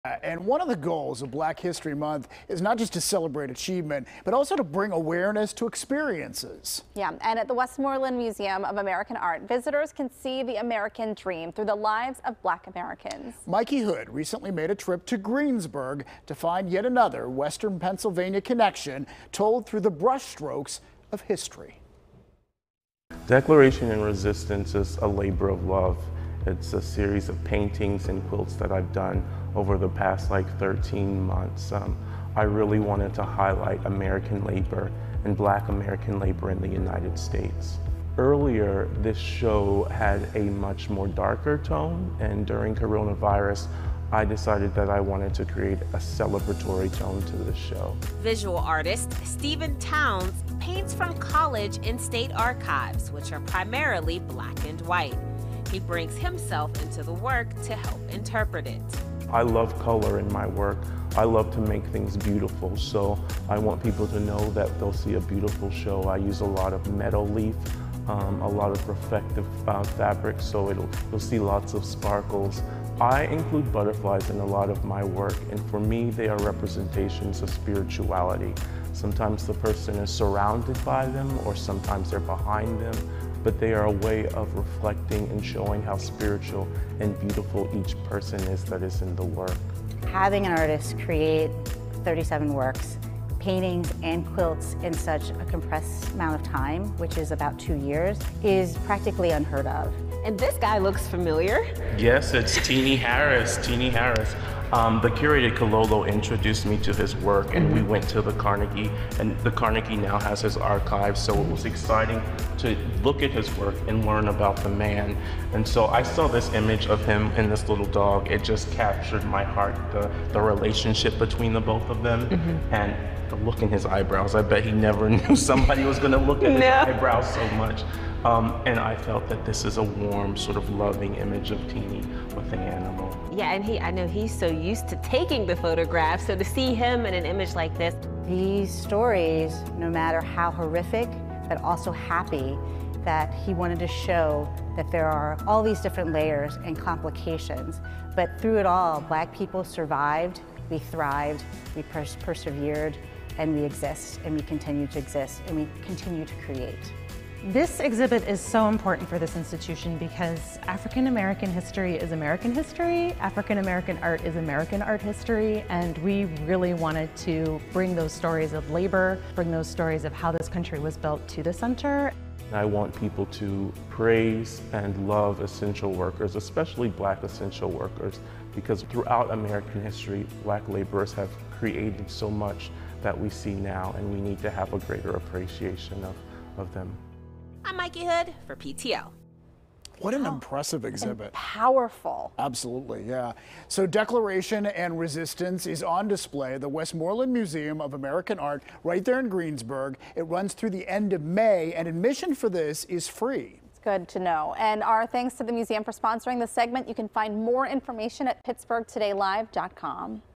Westmoreland Museum Of American Art Opens Black History Month Exhibit
PTL's Mikey Hood visits the Westmoreland Museum Of American Art to check out their Declaration & Resistance Exhibit for Black History Month.